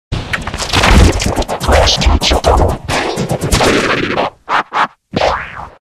Klasky Csupo In G-Major 789 The Original (Changed Pitches)
Made on ‎‎‎W‎ednesday, ‎August ‎19, ‎2020, ‏‎10:49:51 pm to 10:50:07 pm

Note: This is the original G-Major 789 made on Vegas Pro.

Note 2: MediaGuildLogoEditor946 HD VNAUTTP ATHDTC OWN EDCP made up the pitches for me to change its normal audio effect.

Note 3: G-Major 789 effect is improved and changed for the its audio effect because it sounded bland without extra pitches, essential to this effect to make it a true G-Major. You know, having a G-Major effect sound like normal, except if it its a dependent combination resetting it to a normal audio effect (No Effect), doesn't seem to make it a validated effect.

How To Make G-Major 789:

(VideoFX)

1st: Pinch/Punch - Medium Pinch

0
-1.5
-3
-4.5
-6
-7
-8

Effect by MediaAVSEditor835/ DRMAAVE777HDTOT